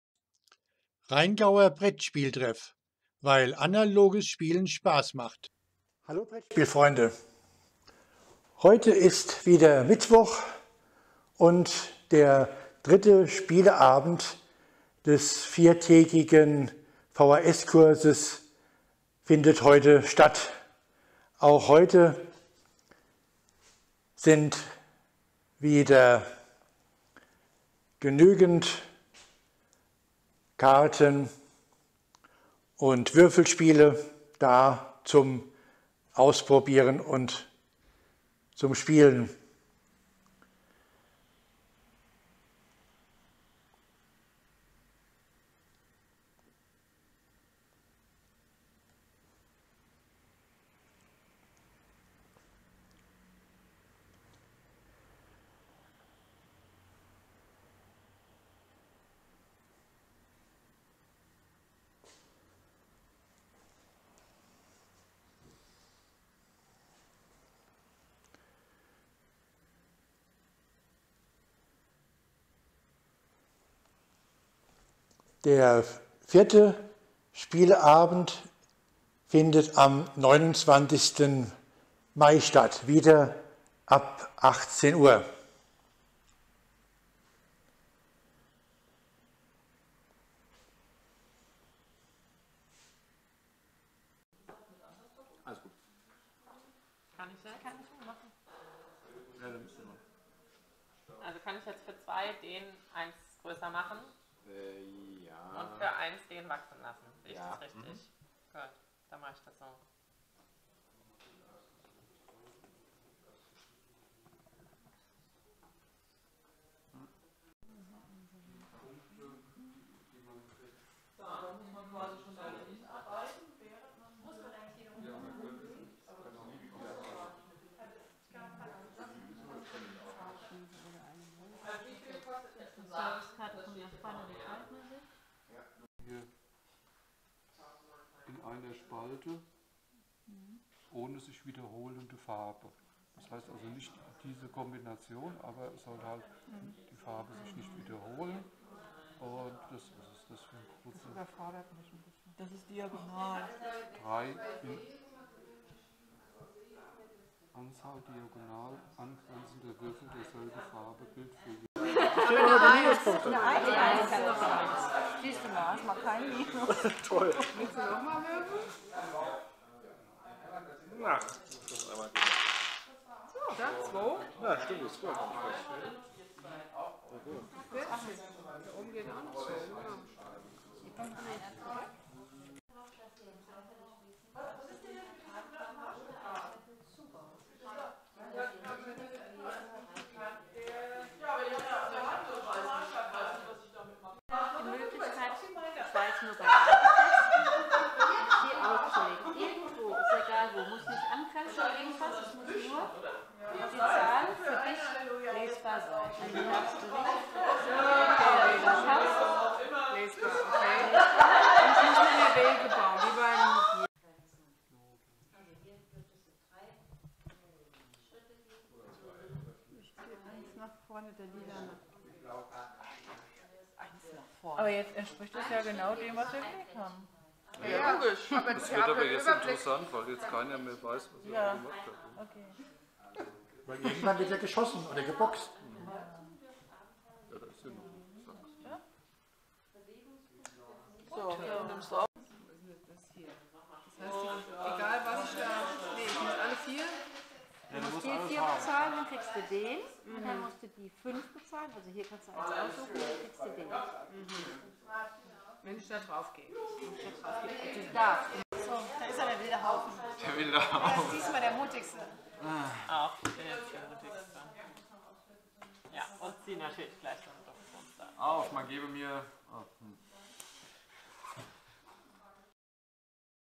3.Spieleabend VHS-Kurs vom Rheingauer Brettspiel Treff
Hier sind ein paar Eindrücke von unserem 3.Spieleabend des VHS-Kurs "Brettspiele". Am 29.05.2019 findet der 4.Spieleabend statt.
Es wurde folgendes gespielt :
Photosynthese, Reykholt, Sagrada, Engel&Bengel, Ohanami, Karube, Colt Express, Istanbul, verflucht und Legendary Forests